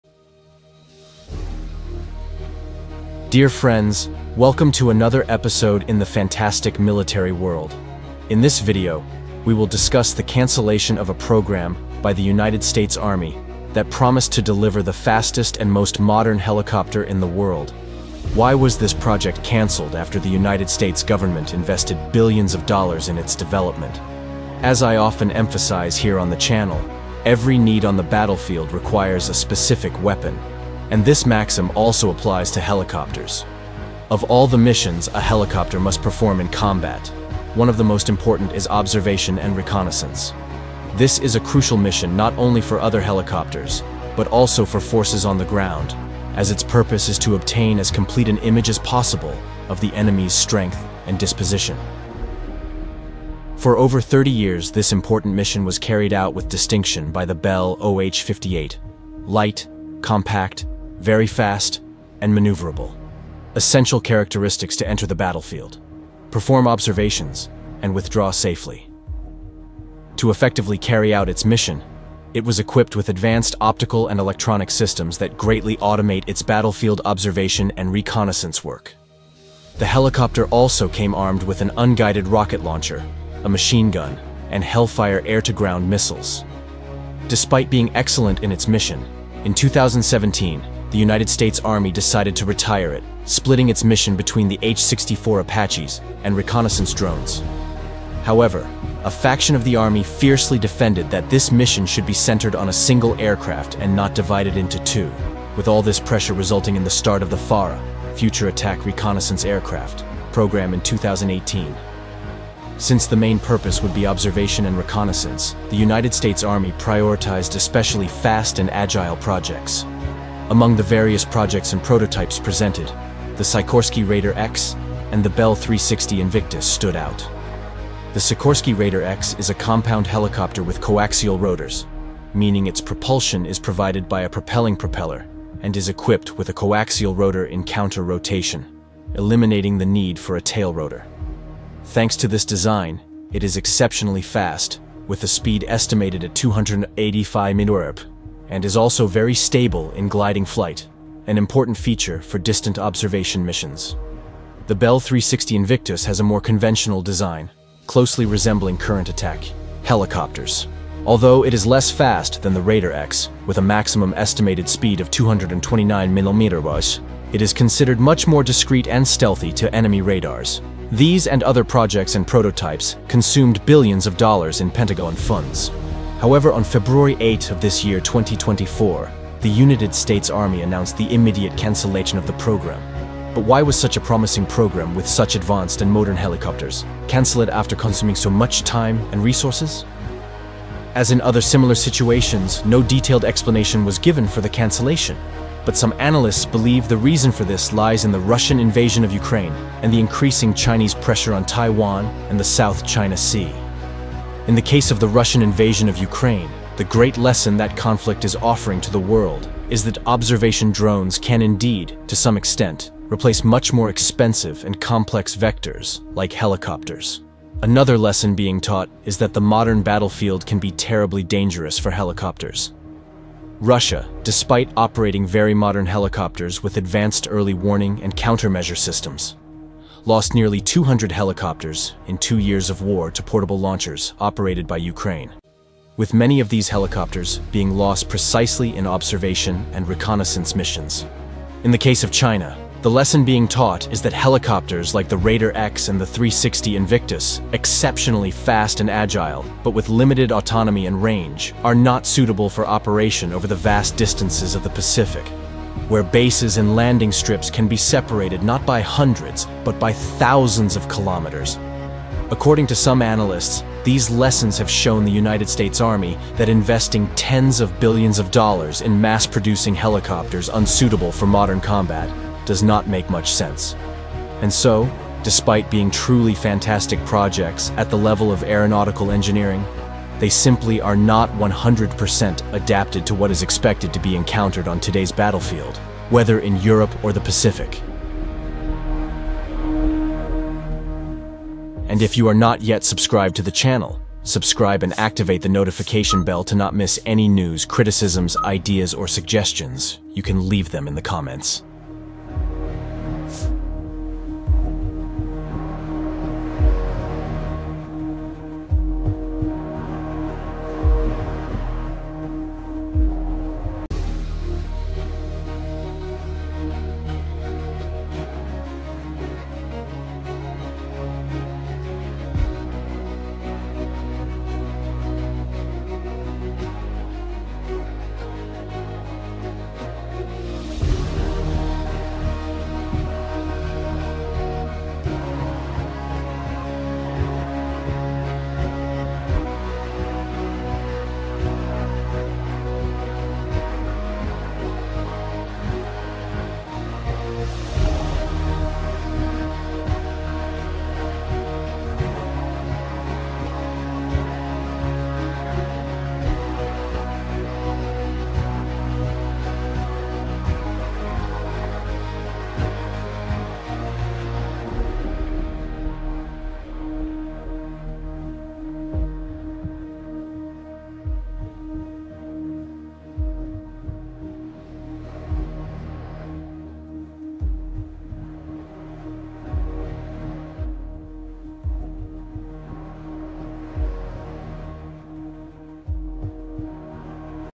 Helicopter of the future canceled after billions of investments – Why did the US Army do this?
Dear friends, welcome to another episode in the fantastic military world. In this video, we will discuss the cancellation of a program by the United States Army that promised to deliver the fastest and most modern helicopter in the world. Why was this project canceled after the United States government invested billions of dollars in its development?